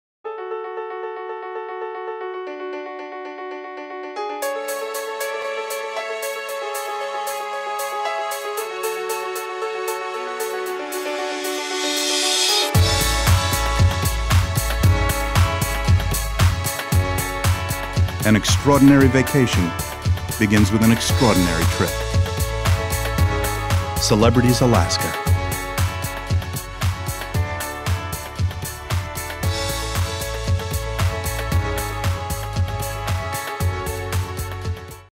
Celebrity Cruises - Alaska
Three of our stunning ships are dedicated to bringing you to all of Alaska’s must-sees in 2017—Celebrity Solstice®, Celebrity Millennium®, and Celebrity Infinity®.

Want a more in-depth encounter with Alaska? We’re introducing two brand new Cruisetours that combine your modern luxury Alaska sailing with a land tour deep into the heart of the Last Frontier.